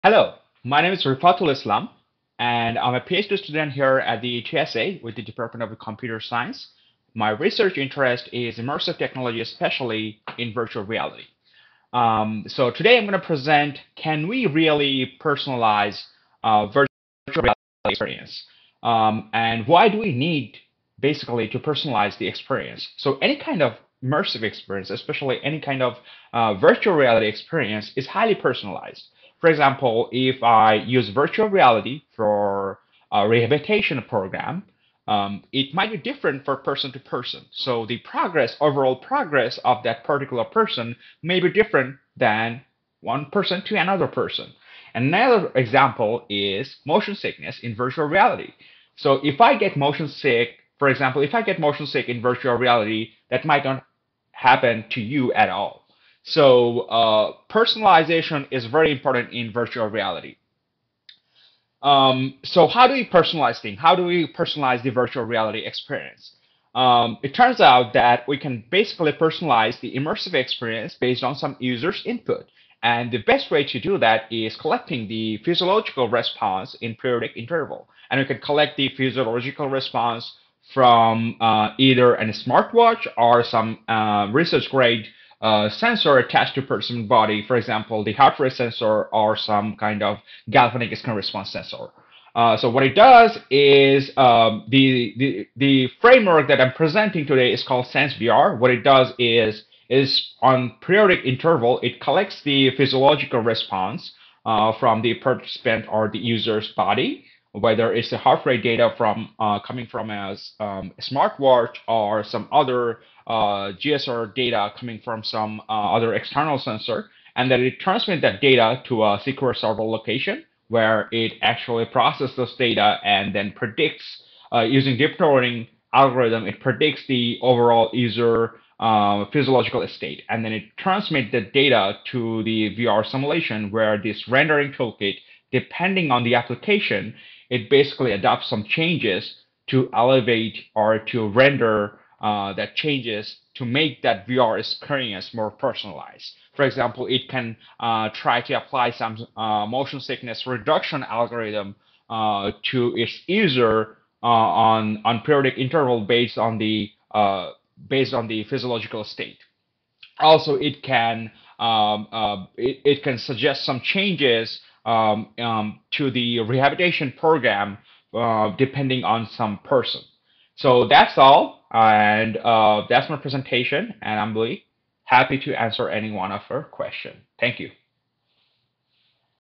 Can Virtual Reality Experience be Personalized? 3MT Research Presentation.
SenseVR: An Adaptive Virtual Reality Framework to personalize Immersive Experience from the user’s physiological state